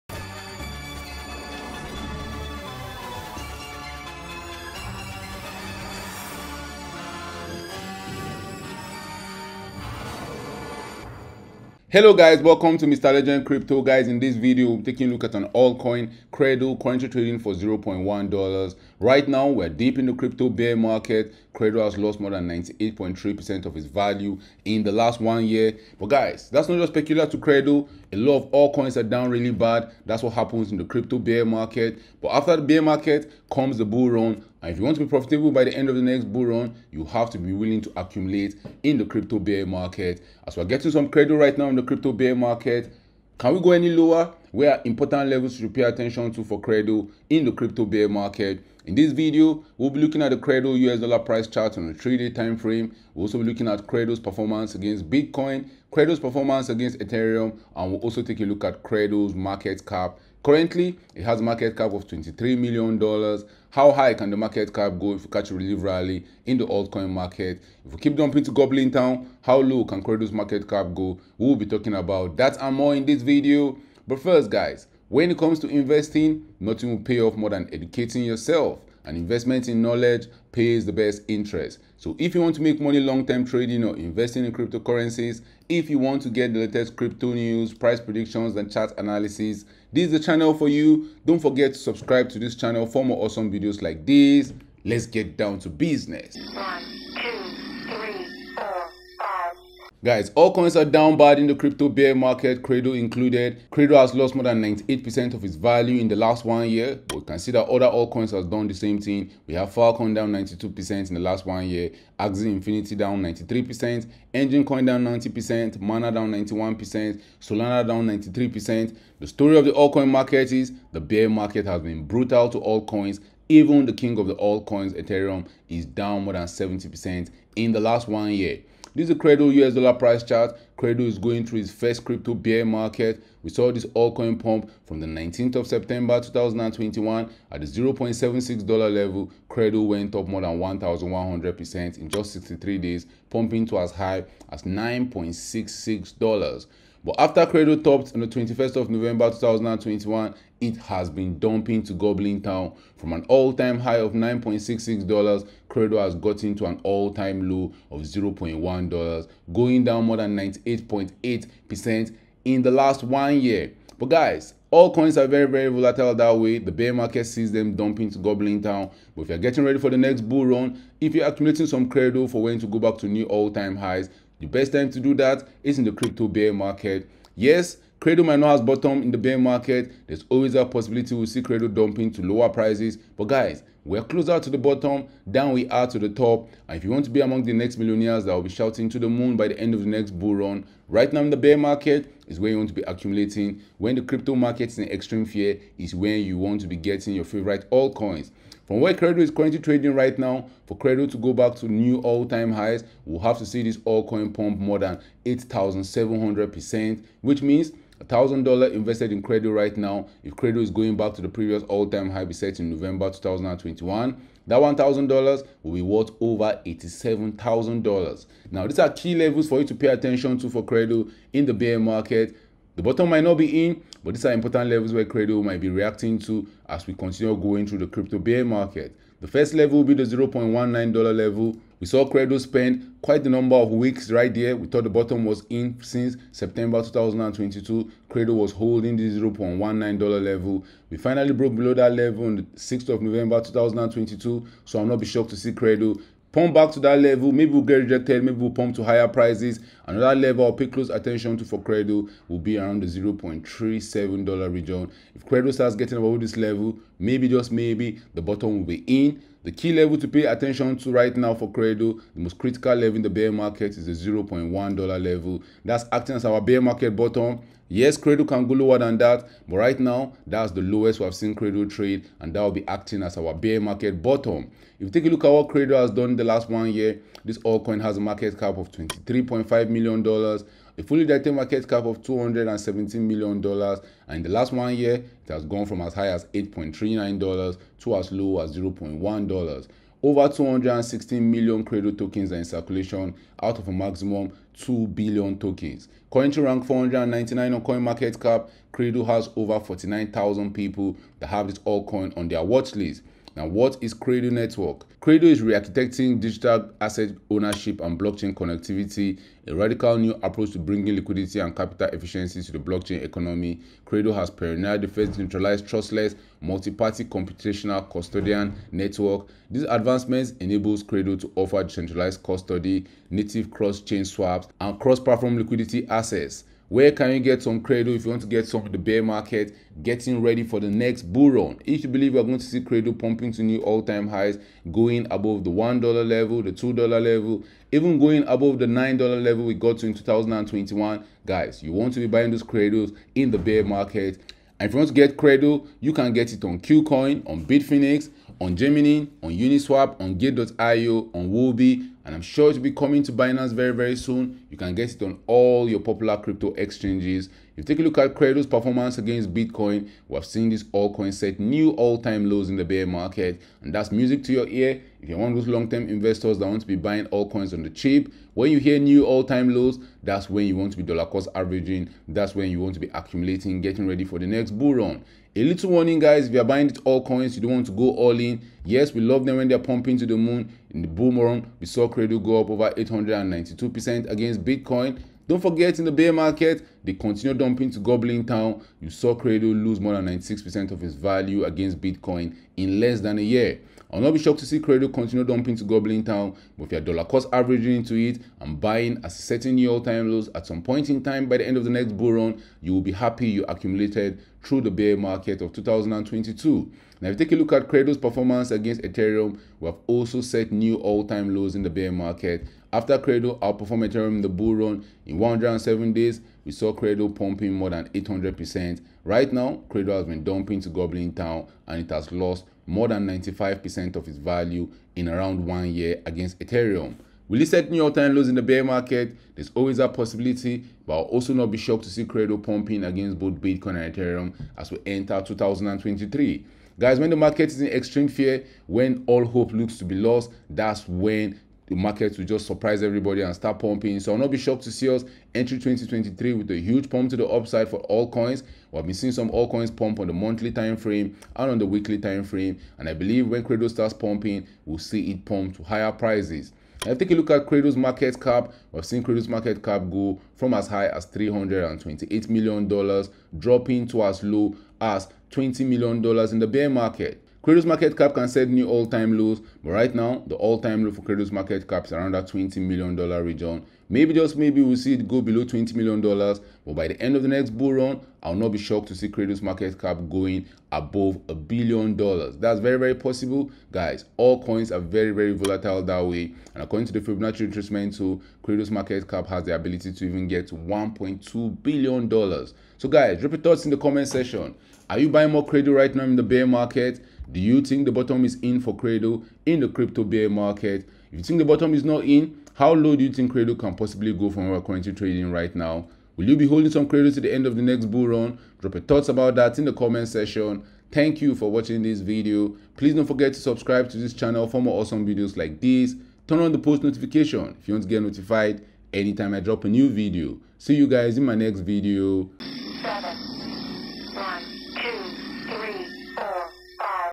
Qredo (QRDO) Bear Market Accumulation. QRDO Price Chart Analysis and Price Prediction 2023
What is the Qredo Network? Qredo is rearchitecting digital asset ownership and blockchain connectivity. A radical new approach to bring liquidity and capital efficiency to the blockchain economy, Qredo has pioneered the first decentralized trustless multi-party computation (MPC) custodial network. This advancement enables Qredo to offer decentralized custody, native cross-chain swaps, and cross-platform liquidity access.

Qredo works at the cutting-edge of cybersecurity and blockchain. By utilizing the latest innovations in cryptography and distributed ledger technology, Qredo delivers a powerful global network for securing and trading digital assets.

MLC